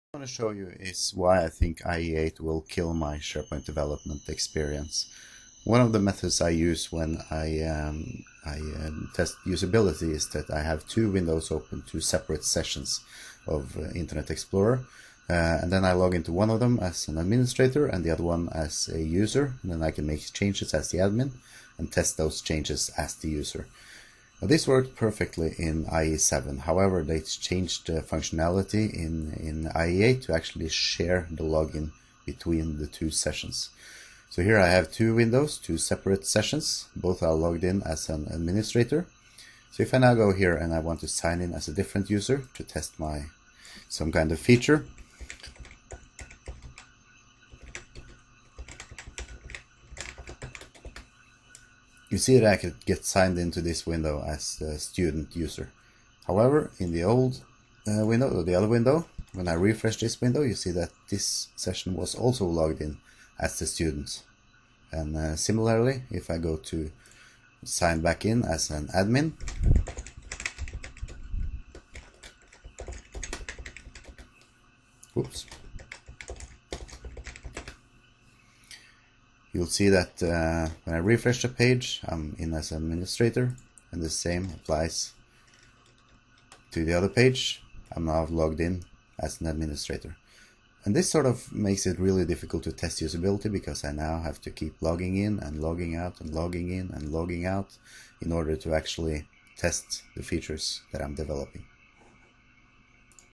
Why IE8 will kill my SharePoint development experience
This video shows why I think IE8 will kill my SharePoint developer experience, especially when developing usability. To learn more on SharePoint, visit
the Understanding SharePoint Journal